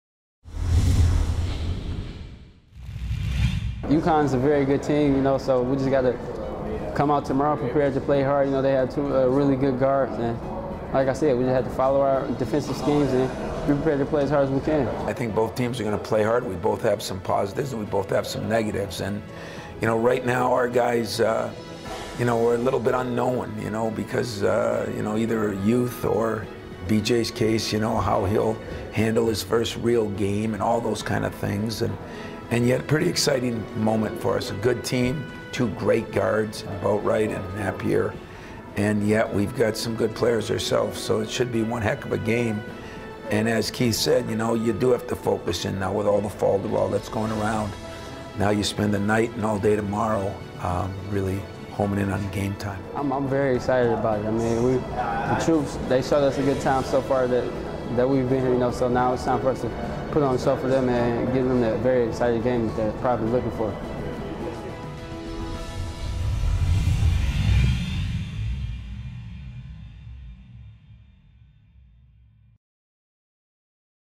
Michigan State Basketball prepares for the Armed Forces Classic
Michigan State Basketball turns their attention to the basketball game against Connecticut for the Armed Forces Classic in Ramstein Germany on November 8, 2012.